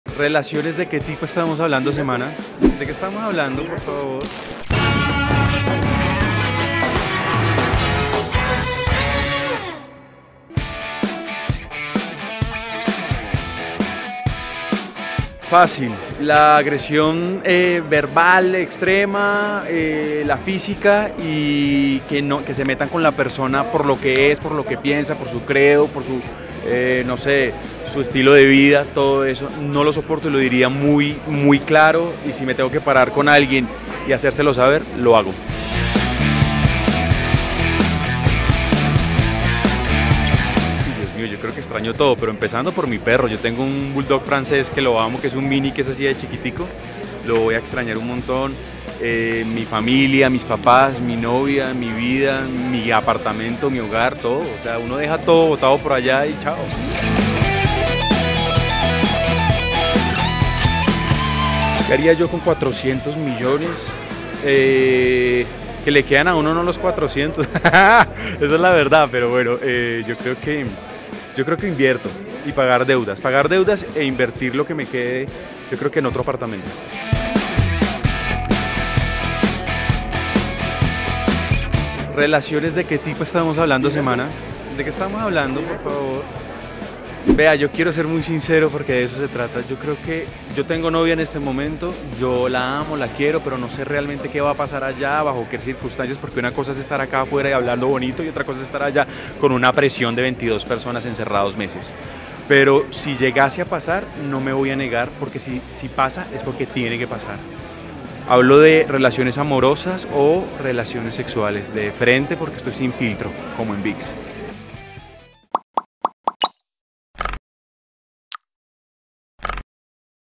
Julian Trujillo y las relaciones dentro de la casa | SEMANA
El actor Julian Trujillo, de ‘La casa de los famosos Colombia’, revela si se atrevería a tener relaciones al interior del reality, “Si llegase a pasar, no me voy a negar”. ¿Qué pensará la novia?